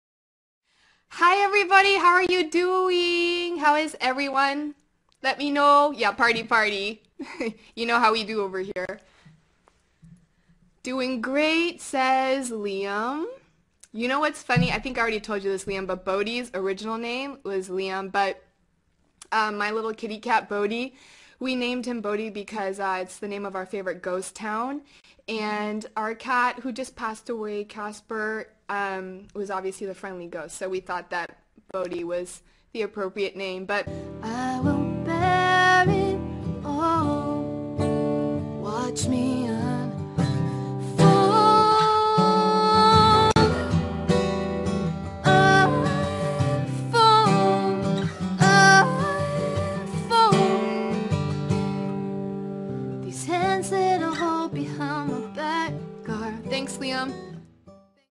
Marie Digby Shout Out - 08/15/2012
This was an awesome live streaming show hosted by Marié Digby.  What makes it great is that I was somehow the #4 top tipper for Marié's fundraiser for her first independent music video!

I have two clips stitched together.

The first part is that she starts off the show having a shout out to me and telling everyone about her new cat's (Bodie) name before he was adopted was "Liam".  How cool is that!

The second part is a shout out from her thanking me for the tips :).